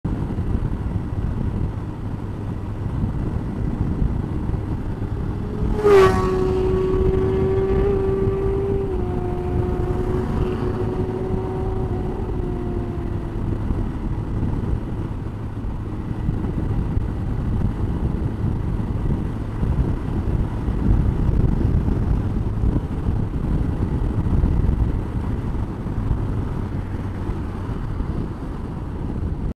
Temporary Australian
I was doing 90KPH what was he doing?